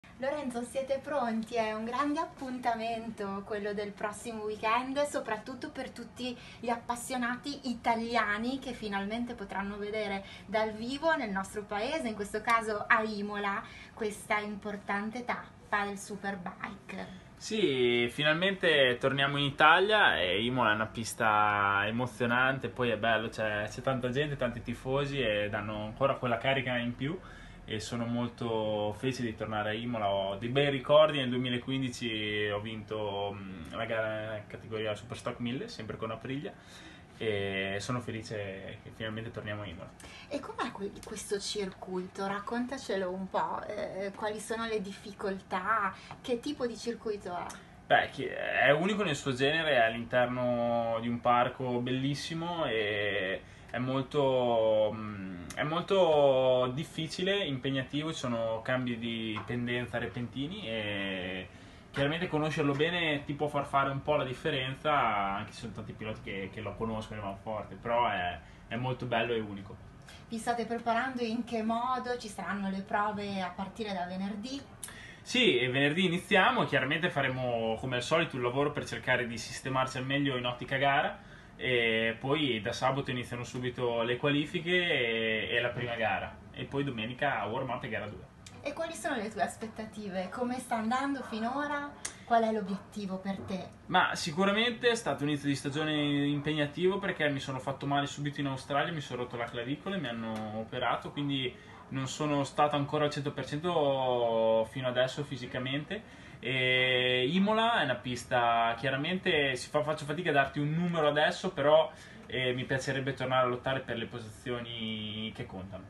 Mondiale Superbike 2018: Lorenzo Savadori presenta la tappa di Imola
Oggi, martedì 8 maggio, in redazione a Radio Bruno a Bologna ospite Lorenzo Savadori, che in sella alla sua Aprila il prossimo fine settimana (11-13 maggio) correrà sul circuito dell'Autodromo di Imola per l'appuntamento con il campionato mondiale Superbike 2018.